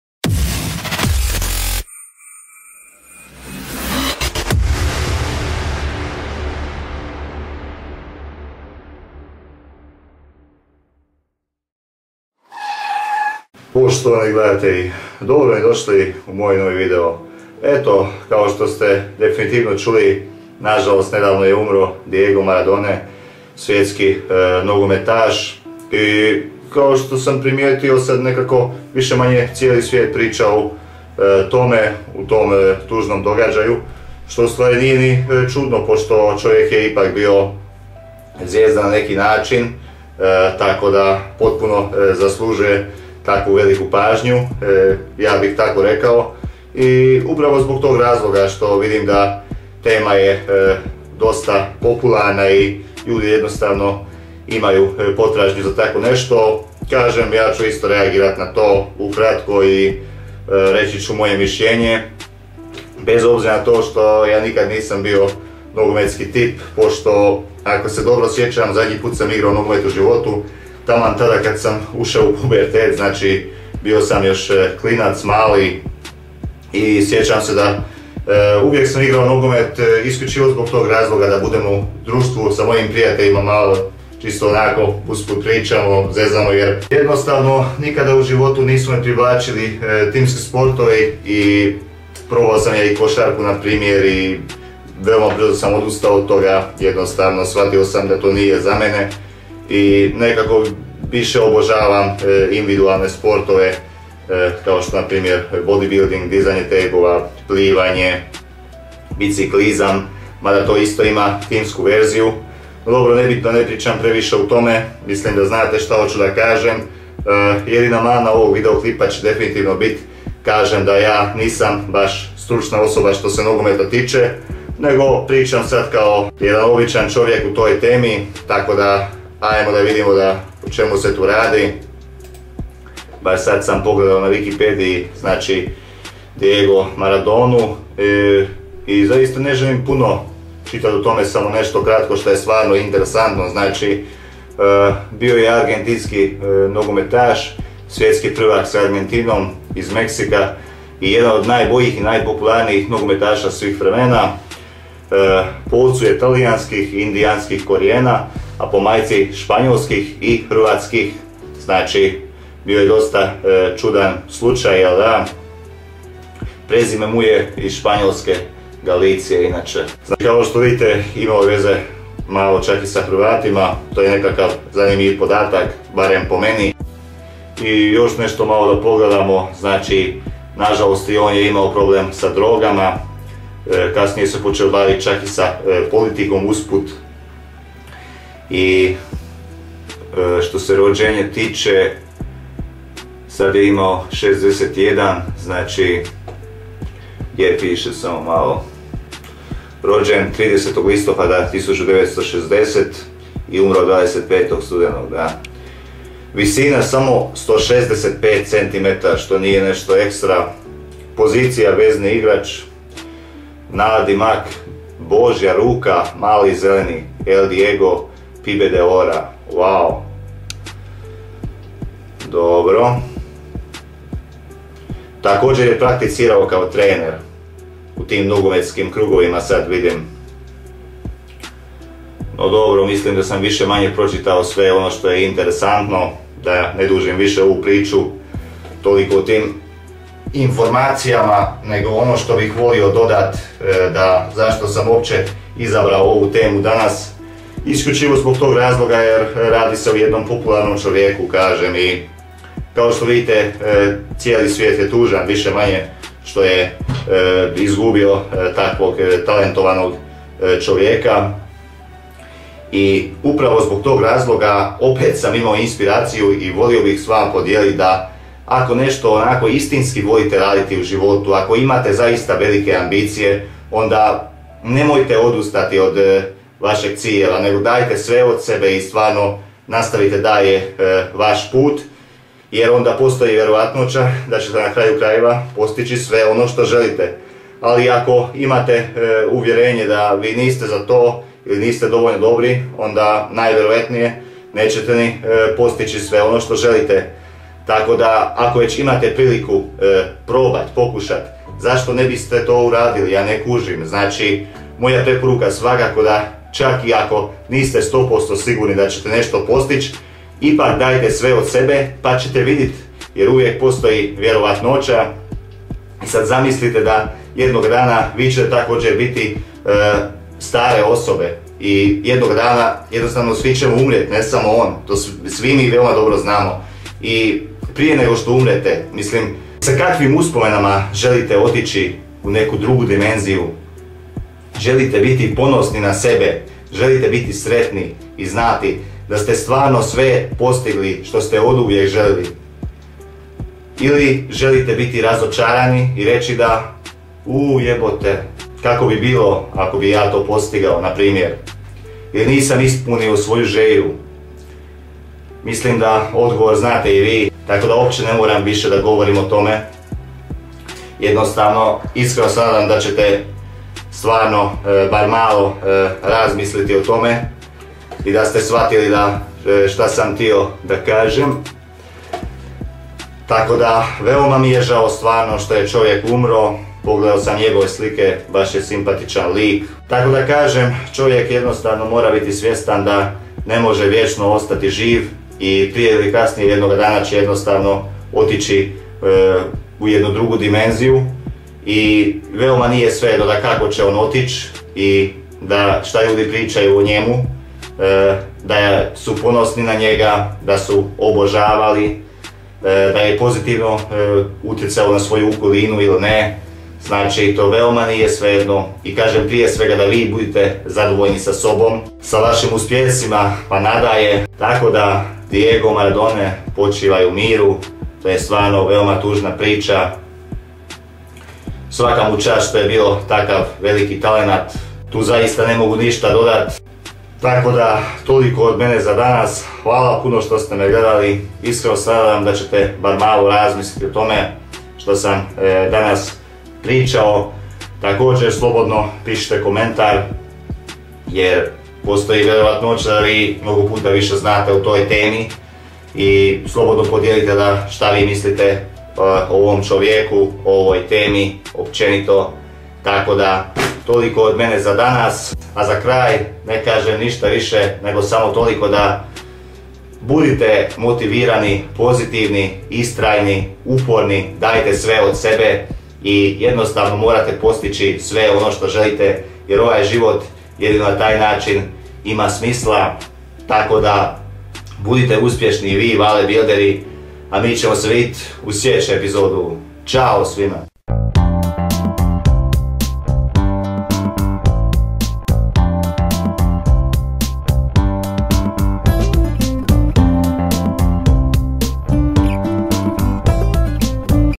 Život DIEGA MARADONE… Šta možemo naučiti?
🧾 INFORMATION ABOUT CLIP:
“Nažalost ponovo smo izgubili jednog velikog nogometaša…
Njegov izuzetno uspješan životni put nek bude primjer svima; da TALENAT TREBA MAXIMALNO ISKORISTITI; i ako odradimo sve što je potrebno onda ćemo definitivno pretvoriti svaku našu želju u realnost!
Počivaj u miru: DIEGO MARADONA! ☹”

Citat by: Vale Bro
Kontakt-informacije / ako ste zainteresirani za personalni trening ili da reguliram vaše prehrambene navike (uživo/online):
Instagram profil: @vale_building
Facebook stranica